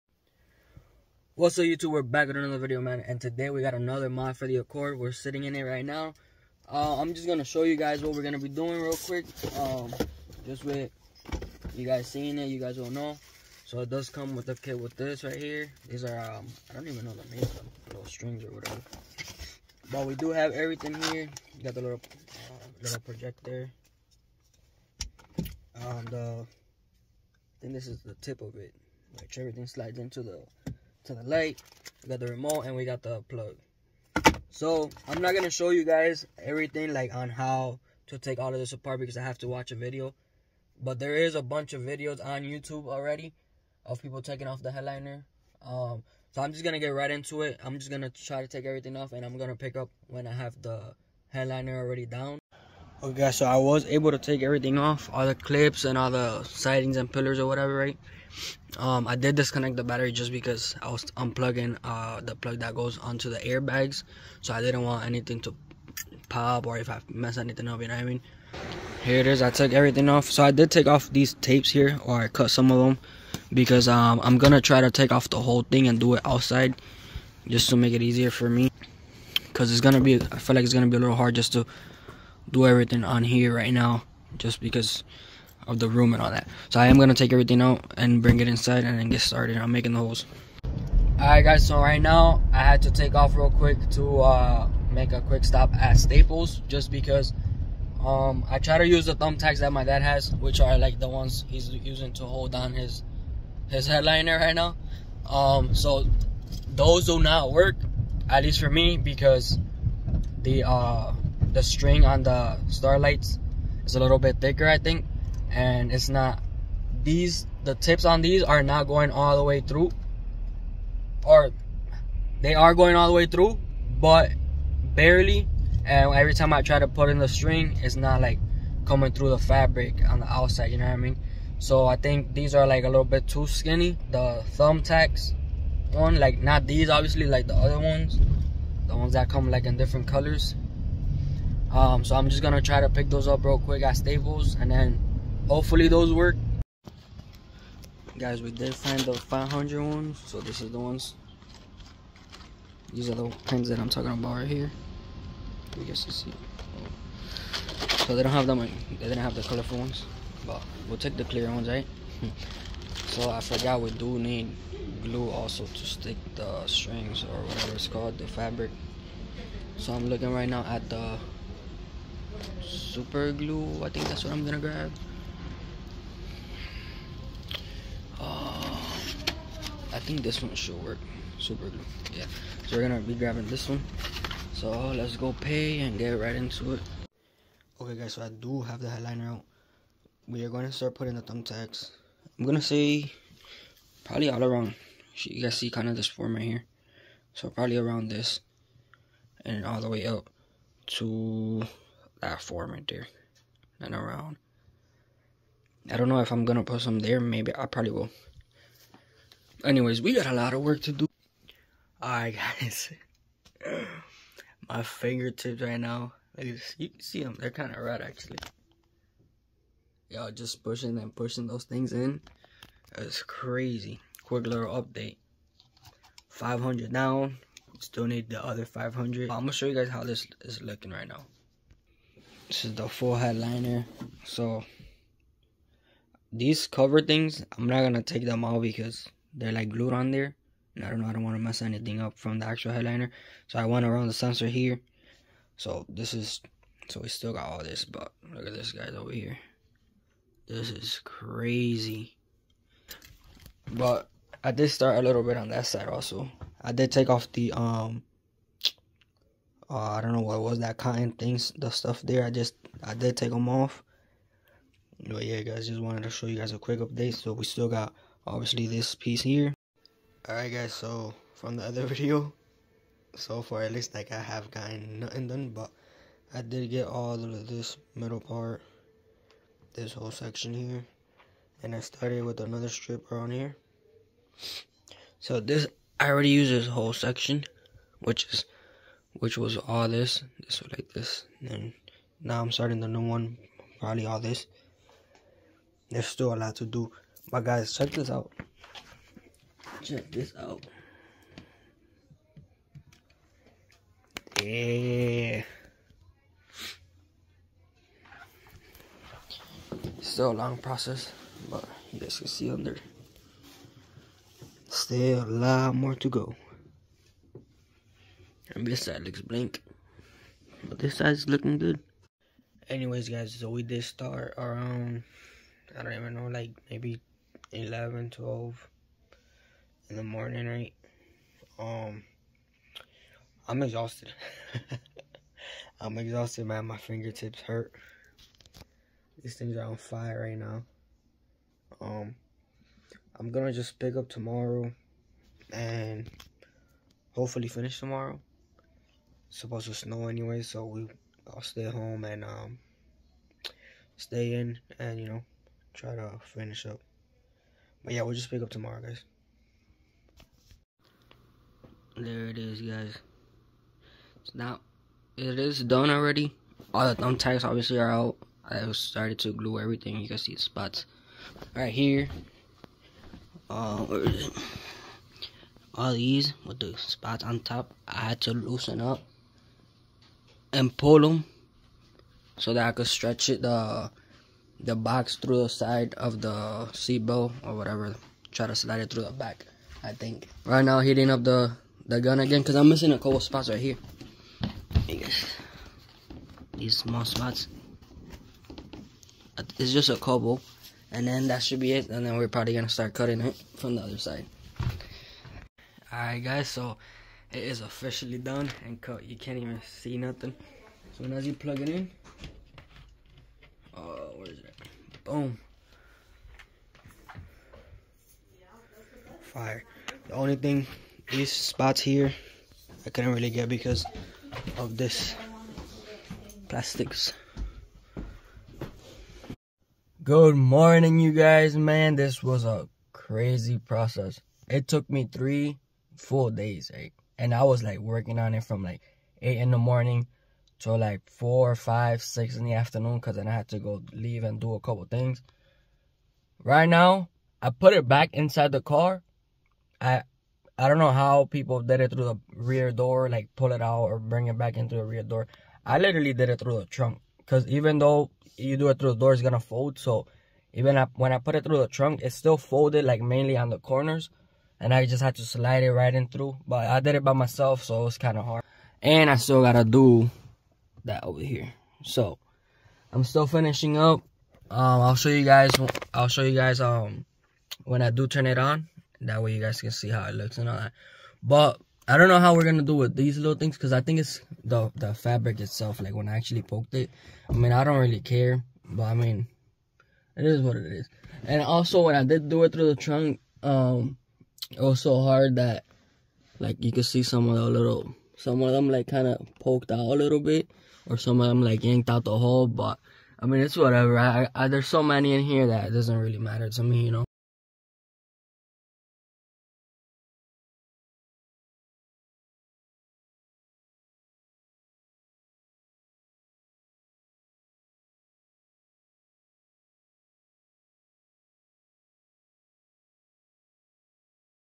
Starlight headliner on 10th gen accord
This process was not an easy one but it was definitely worth it I'm even more in love and that's just the beginning of the mods but its definitely a game changer for sure I recommend you to do it for sure again its not easy but its worth it, any questions feel free to ask thank you guys don't forget to like comment and subscribe for more mods